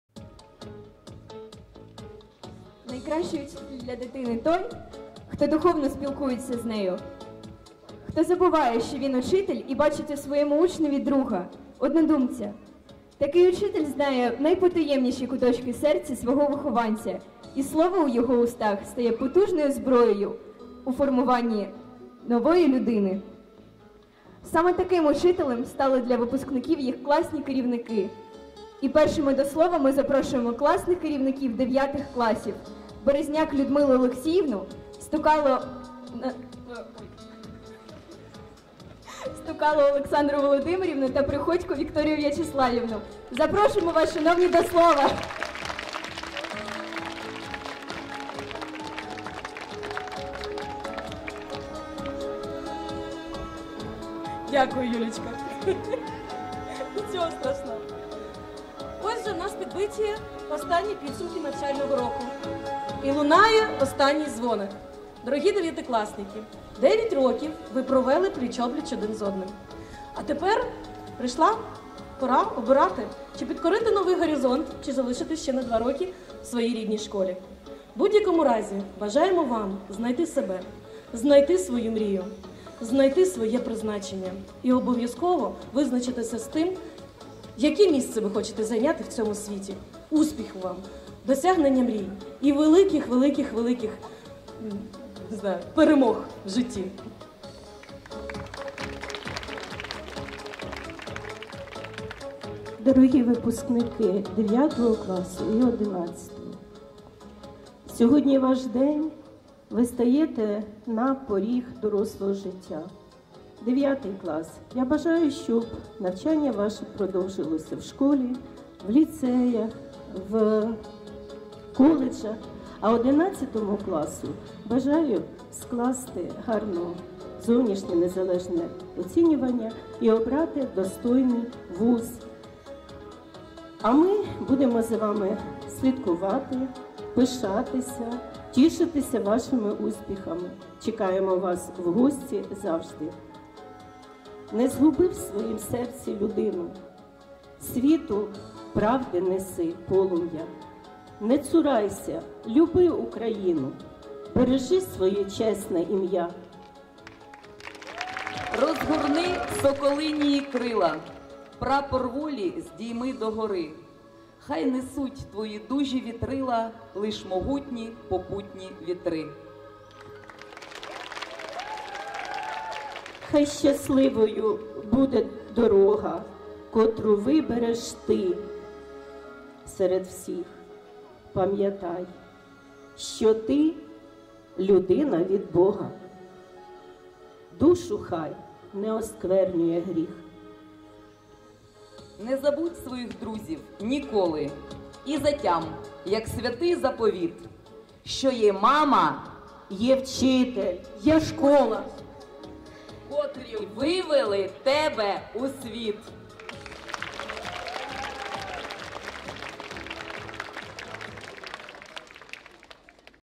Свято Останнього дзвоника. Звернення класних керівників 9-тих класів (26 травня 2021)
На правах вільного використання та поширення